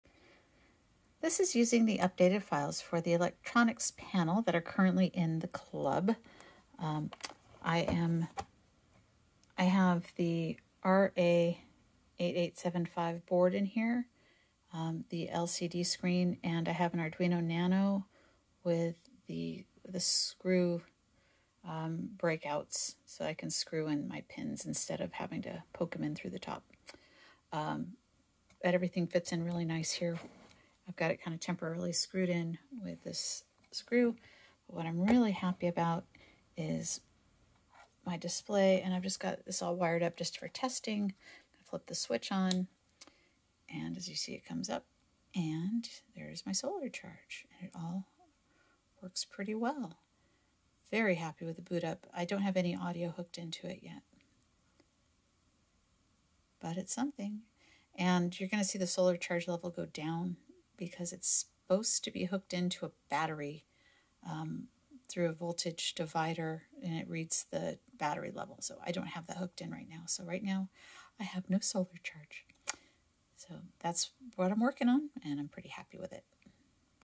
Wall-E Electronic Panel Box - LCD Solar Charge Display Test
Finally finished the bulk of the wiring and mounting for Wall-E's Solar Charge Display Panel.  I was completely out of my comfort zone with this, but I made it all work! I still have lots of weathering to do on this part, but happy with things so far. 😉

Many thanks to the builders at the Wall-E Builder's Club for their help!